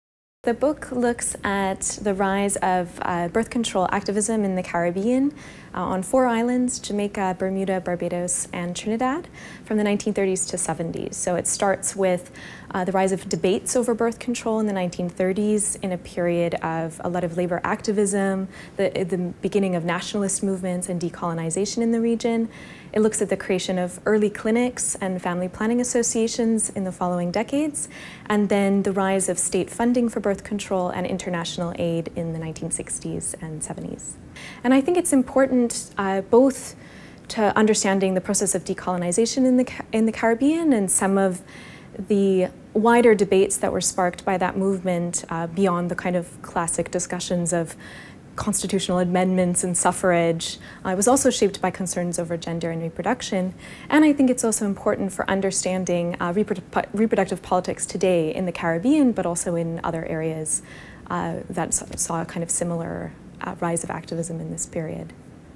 Insight | Charting the history of birth control in the Caribbean
Nicole Bourbonnais, Assistant Professor of International History, introduces her new book, "Birth Control in the Decolonizing Caribbean. Reproductive Politics and Practice on Four Islands, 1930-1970"